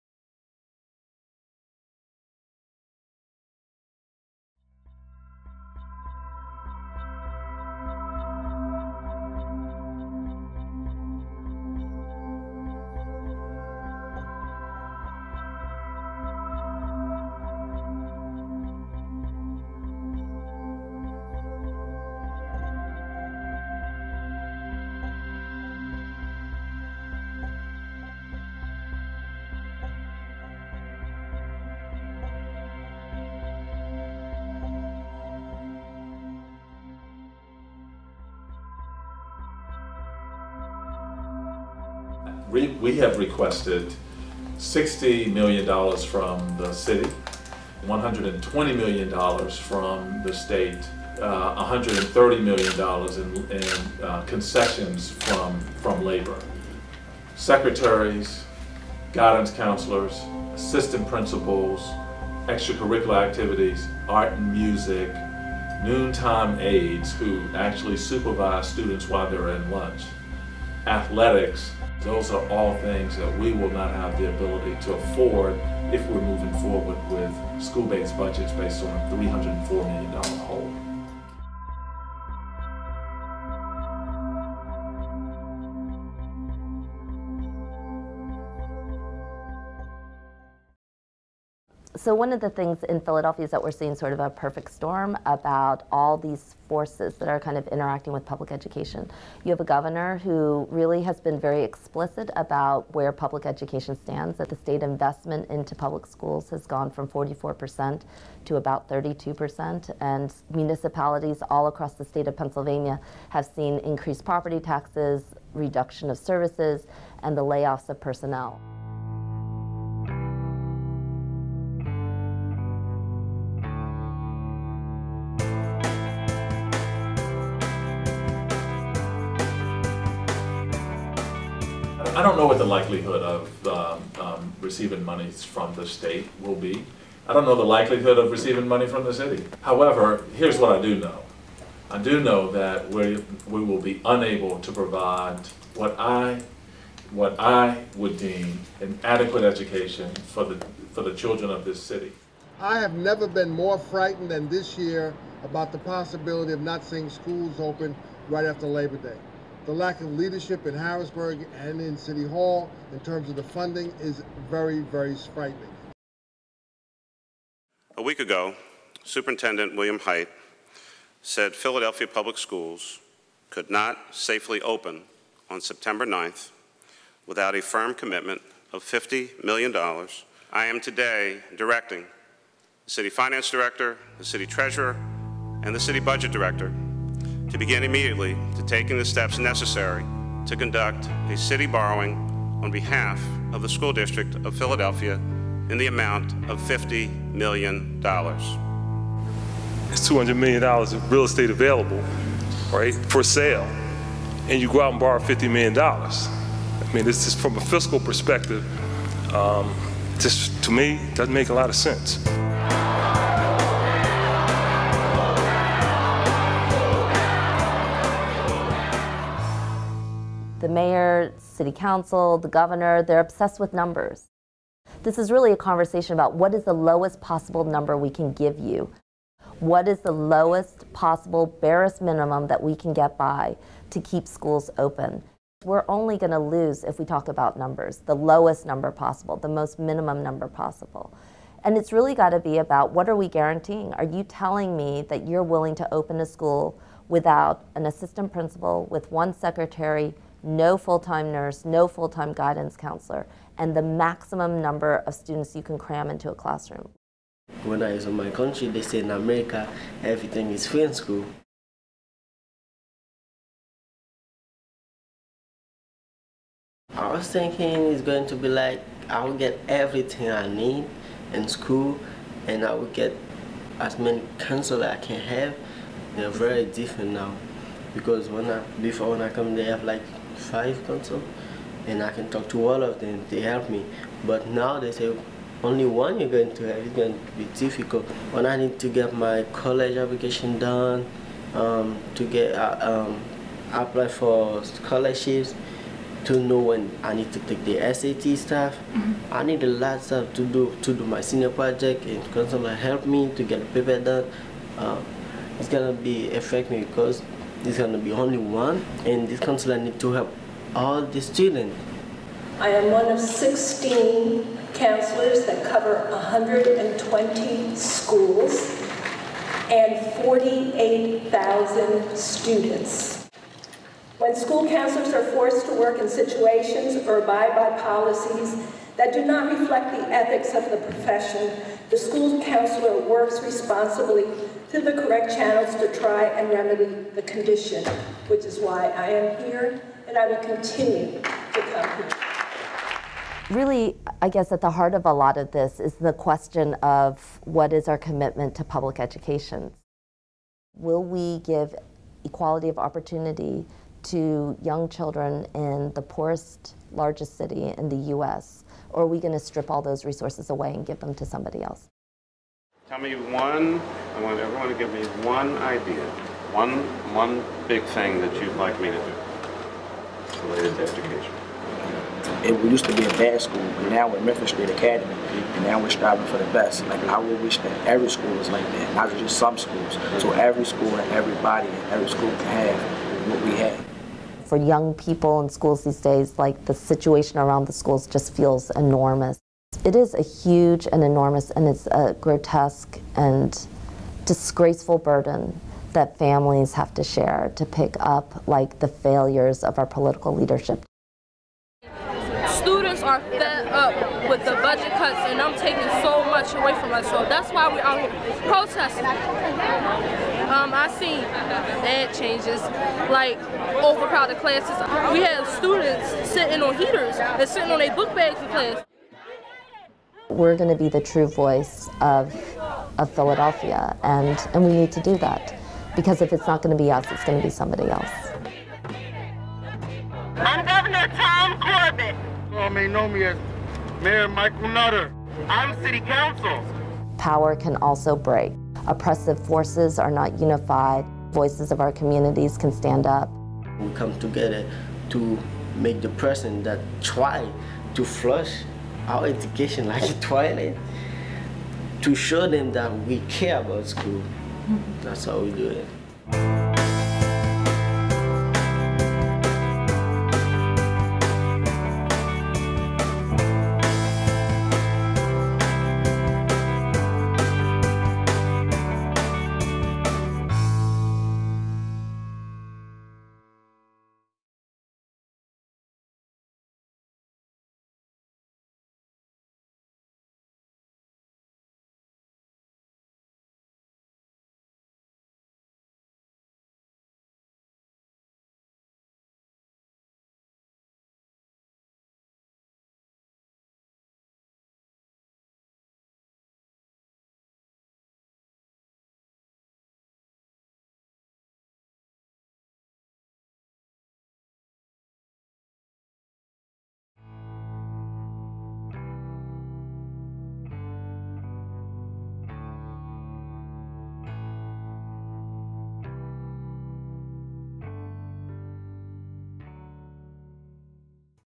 The Fight for Education
An Al Dia Documentary Op-Ed

The Pennsylvania state constitution states that, "The General Assembly shall provide for the maintenance and support of a thorough and efficient system of public education." Yet in Philadelphia, where one out of three students comes from a low-income household, education is severely underfunded.

"The Fight for Education" is a documentary op-ed that explores the 2013 efforts of teachers, students, parents and the community to achieve equal access to education in the city of Philadelphia.

The Speakers:
Helen Gym is a Philadelphia parent and founder of Parents United for Public Education, a citywide organization that focuses on accountability to ensure that the district budget is distributed to the classroom.

Henry Pierre-Louis, a senior at South Philadelphia High School and a member of Philadelphia Student Union, moved from Haiti to Philadelphia three years ago. Through his involvement in Philadelphia Student Union, he has participated in conferences across the United States and protests in the city of Philadelphia to achieve a safe and quality education.

Sharron Snyder is a senior at Benjamin Franklin High School and an active member of Philadelphia Student Union.

Dr. William Hite has been Philadelphia School District's Superintendent since June 2012.